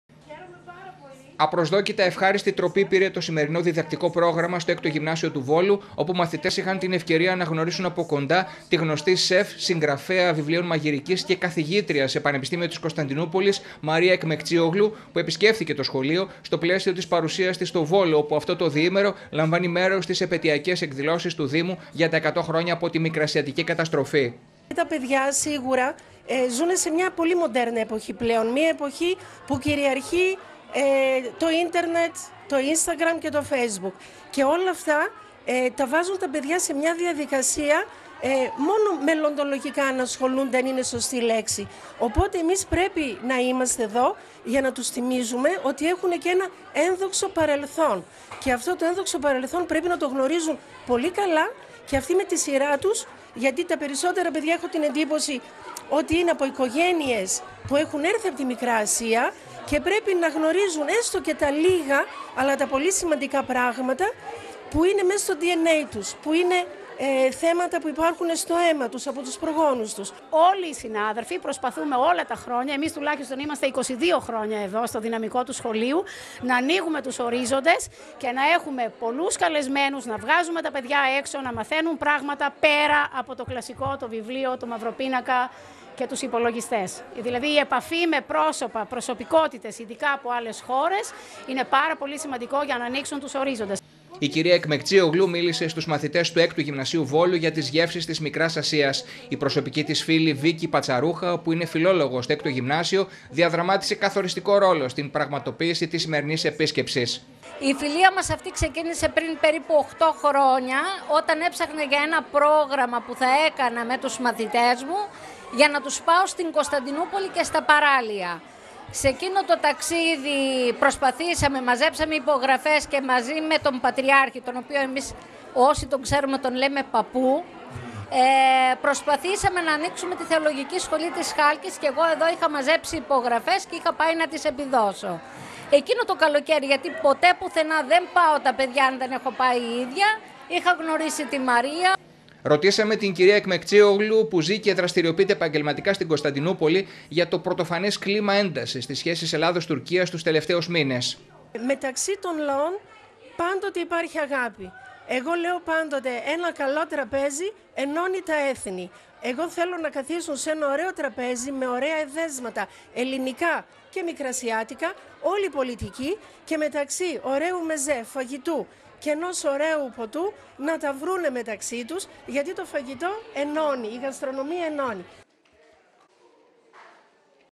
Βόλος Στο 6ο Γυμνάσιο Βόλου η Μαρία Εκμεκτσίογλου 150922
Η γνωστή σεφ και συγγραφέας βιβλίων μαγειρικής, Μαρία Εκμεκτσίογλου, που βρίσκεται από χθες στον Βόλο, στο πλαίσιο των εκδηλώσεων μνήμης του Δήμου για τη συμπλήρωση 100 χρόνων από τη Μικρασιατική Καταστροφή, επισκέφθηκε νωρίς το μεσημέρι στο 6ο Γυμνάσιο και συνομίλησε με τους μαθητές και τους εκπαιδευτικούς για τις γεύσεις και τις γαστρονομικές απολαύσεις της Μικράς Ασίας. Μάλιστα, όπως είπε, απαντώντας σε ερώτηση της TRT, με αφορμή τη συνεχιζόμενη κλιμάκωση της έντασης στις σχέσεις Ελλάδας και Τουρκίας, οι δύο λαοί δεν έχουν τίποτα να χωρίσουν και πως γύρω από ένα τραπέζι με λαχταριστά εδέσματα όλα τα προβλήματα μπορούν να συζητηθούν και να επιλυθούν